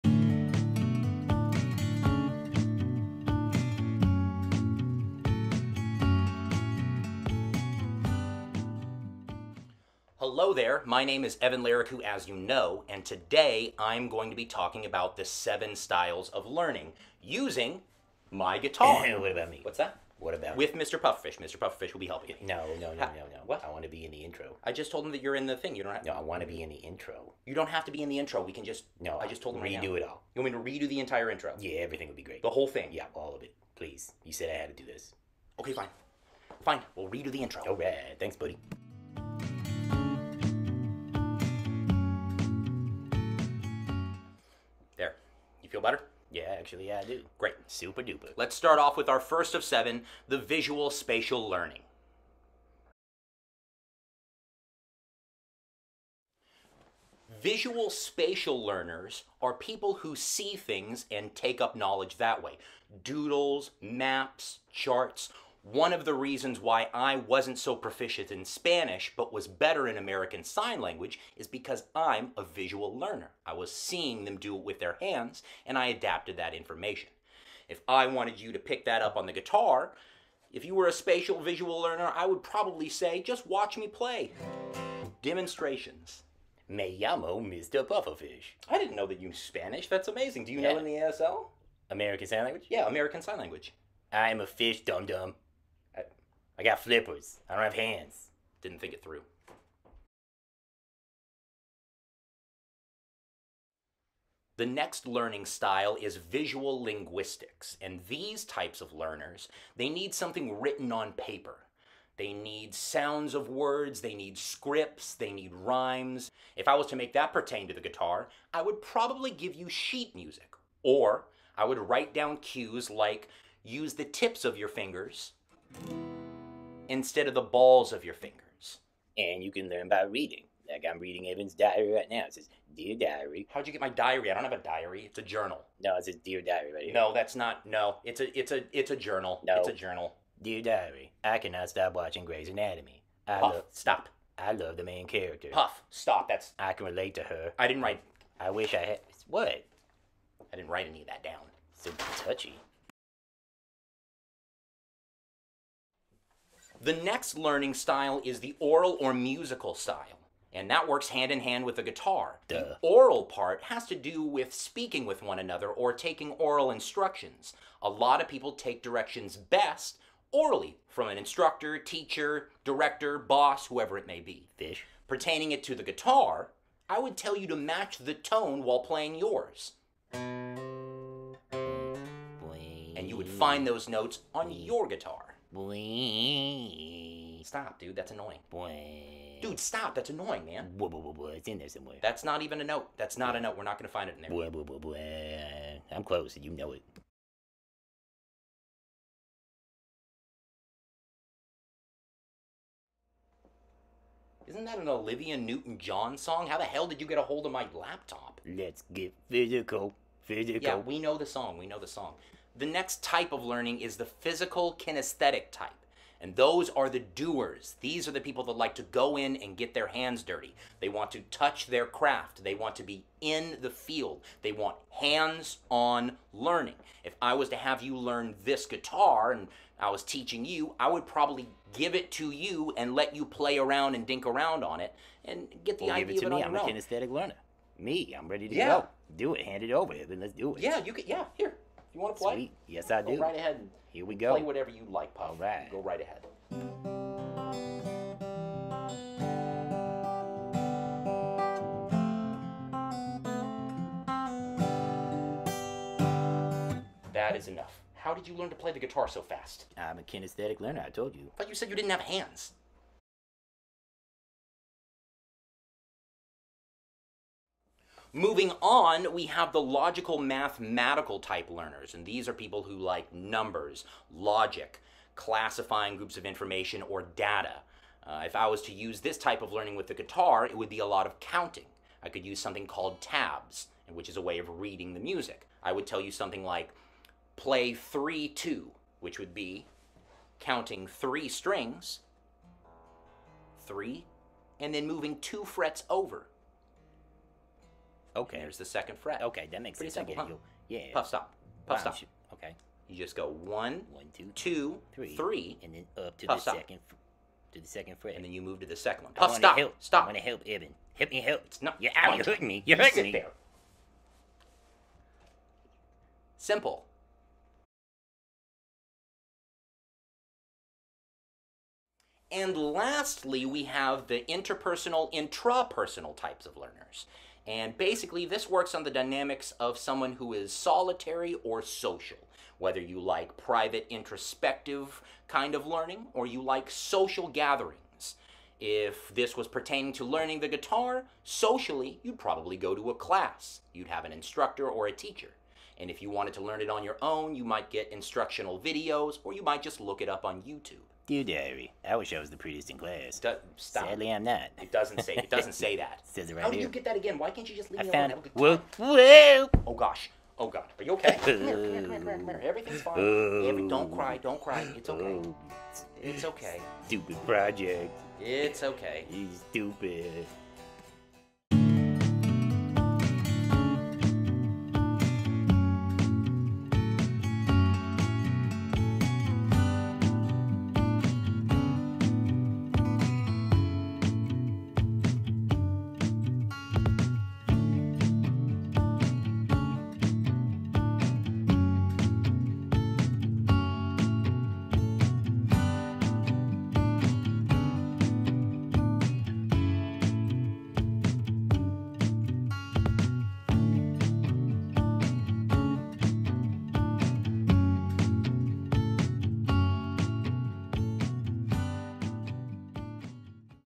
The 7 Learning Styles
Pufferfish and I teach you the 7 learning styles!